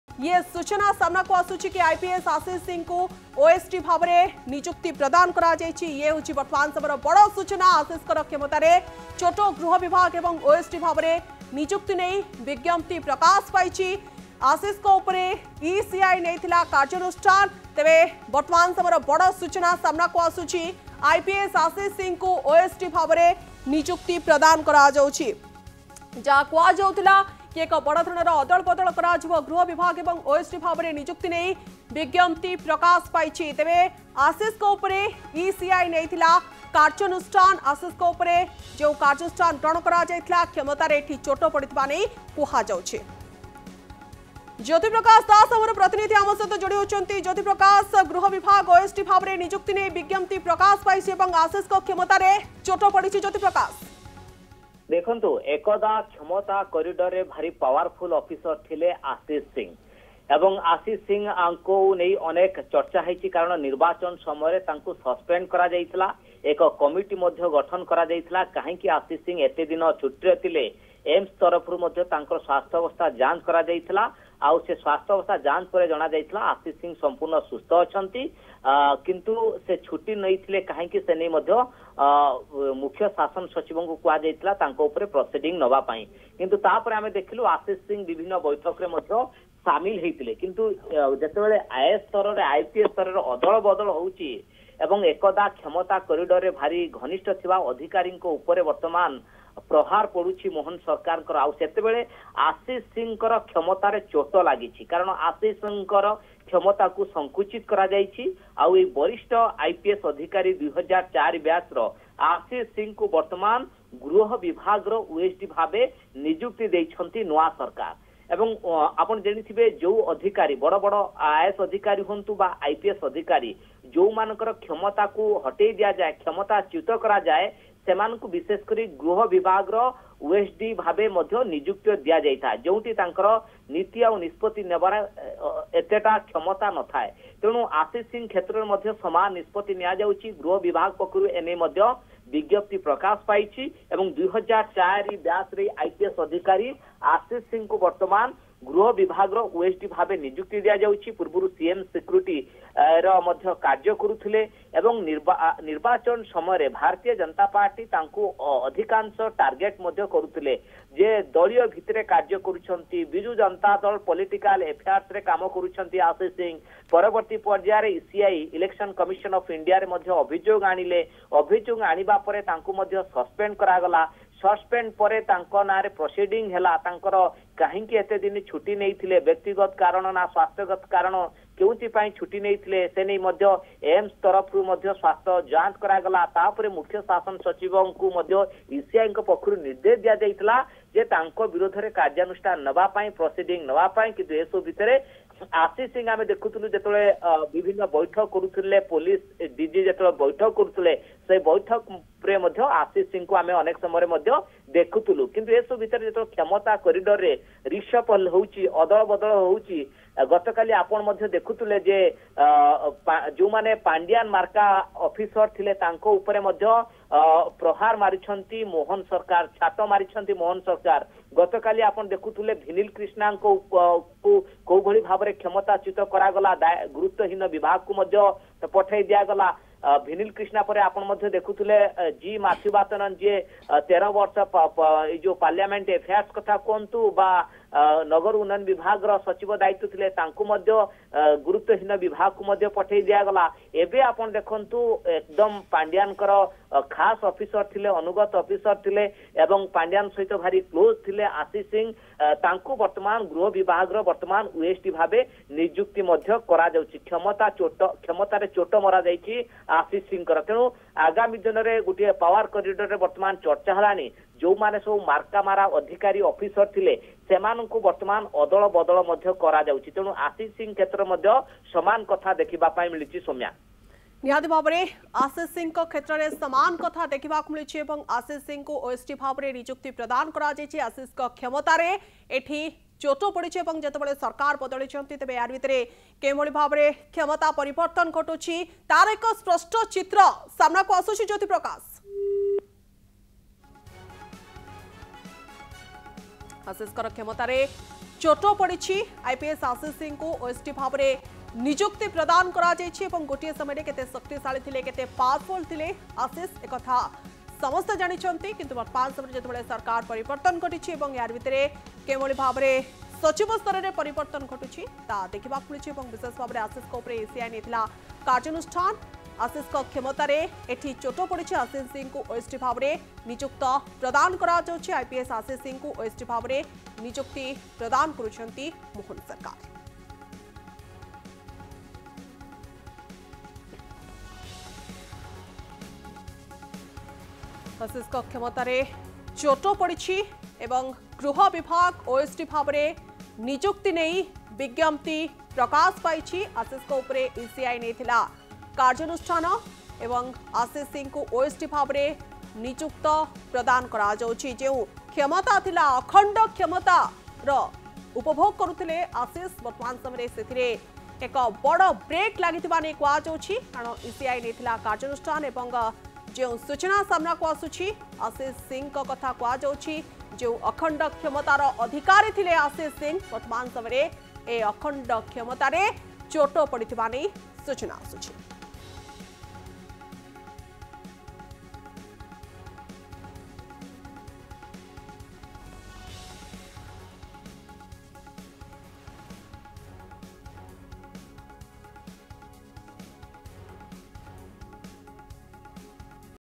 IPS Ashish Kumar SIngh Is Transferred & Posted As OSD Of Home Department
IPS Ashish Kumar Singh has been transferred and appointed as the Officer on Special Duty (OSD) in the Home Department